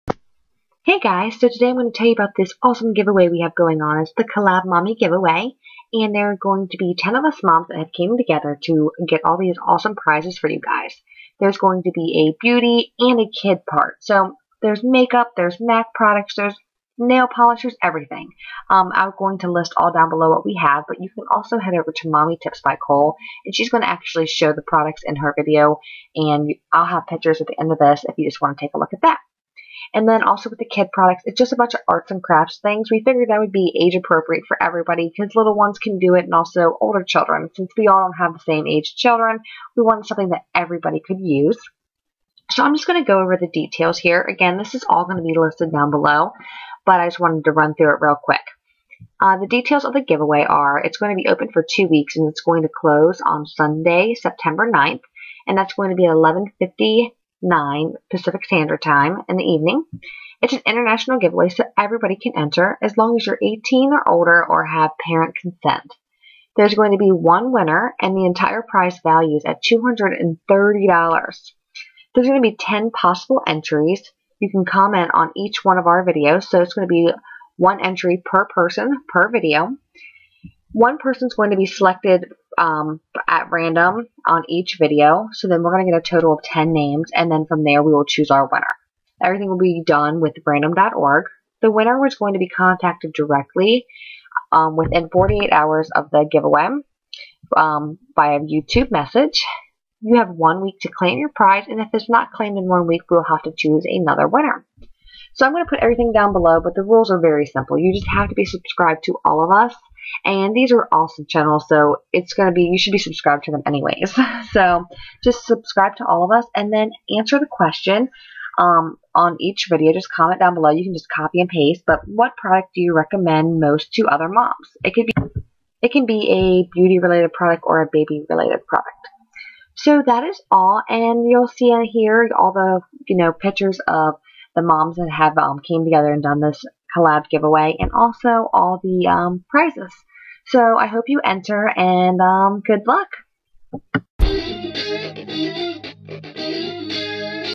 COLLAB MOMMY GIVEAWAY !!! (CLOSED)
~♥~♥~♥~♥~   OPEN ME  ~♥~♥~♥~♥~
DETAILS:
-Giveaway open for 2 weeks and will close on Sunday September 9, 2012 @11:59pm PST
-Must be 18 or have parental consent
-One winner (entire prize valued over $230!!!)
-10 possible entries
-One entry per person/per video
-One person will be selected at random per video and the 10 names will be entered into a randomizer via random.org and one winner will be chosen.
-Winner will be contacted directly within 48hrs after the close of the giveaway, via You Tube message and has one week to claim prize. If not claimed within one week, another winner will be selected at random.

RULES:
What product do you recommend most to other moms? (It can be beauty or baby related)
...you can copy/paste the same answer on each video entry

(Mommy prizes include...)
-MAC limited edition blush "supernova"
-MAC limited edition eyeshadow "earthly"
-MAC limited edition creamsheen lipstick "peachy blossom"
-MAC mini fix plus
-Urban Decay SMOKED Palette
-Tokidoki rollerball perfume "donutella"
-Tokidoki mini shadow palette "lion poppa"
-Real Techniques 5 piece eye brush set + case/stand
 -Essie nail polish "ladylike"
-Seche Vite top coat
-EOS lip balm "strawberry sorbet"
(Kid pizes include...)
-Crayola washable kids paint (6pc)
-Crayola assorted paint brushes (5pc)
-sponge brushes (3pc)
-double-sided lap board
-dry erase markers (2pc)
-magnetic mini erasers (2pc)
-Crayola colored pencils (12pc)
-Crayola washable markers (8pc)
-Crayola crayons (24pc)
-Crayola washable sidewalk chalk (4pc)
-assorted design pencils (10pc)
-pencil snap-its (3pc)
-rubber stamps (4pc)
-red ink pad
-blue ink pad
-Elmer's school glue
-foam alphabet & number stickers (100+pc)
-foam people & animal sticker (100+pc)
-adhesive wiggly eyes (75pc)
-handprint design container for art supplies
-foam penant/flag shapes (10pc)
-foam handprint shapes (10pc)
-pom poms (50pc)
-novelty fuzzy sticks (20pc)
-craft sticks (60pc)
-award bookmarks (24pc)
-holographic correcting stickers (210pc)
-EZ-Freeze energy sandwich container
-EZ-Freeze energy snack container
-Hostess Cupcakes snack-tainer

FTC: All these products were purchased with our money